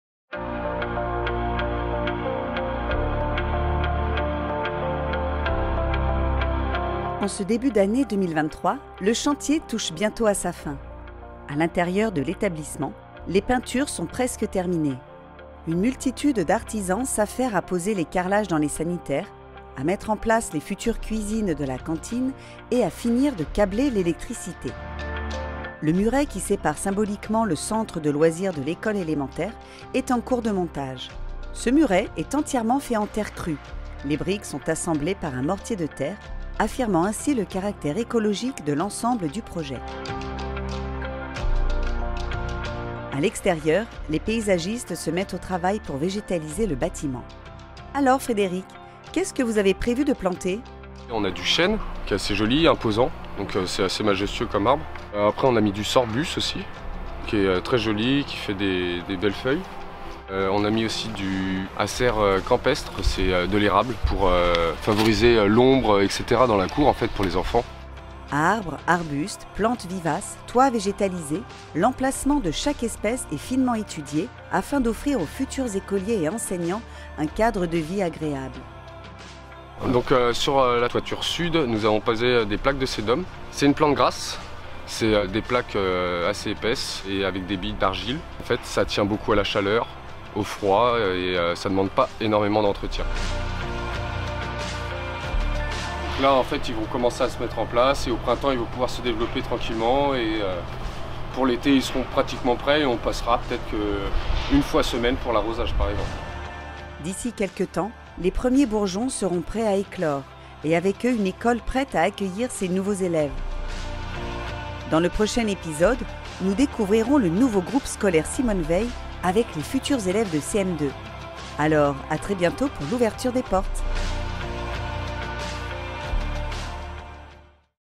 Nouveau Groupe Scolaire à Bussy-Saint-Georges #6
6è épisode de notre suivi du chantier du groupe scolaire Simone-Veil. Un chantier qui touche bientôt à sa fin ! Dans cet épisode, Frédéric, paysagiste, nous présente les différentes essences d'arbres, arbustes et plantes vivaces qui y sont plantées pour offrir aux futurs élèves et enseignants de l'école un cadre de vie agréable.

Vidéo réalisée par Chlorofilm.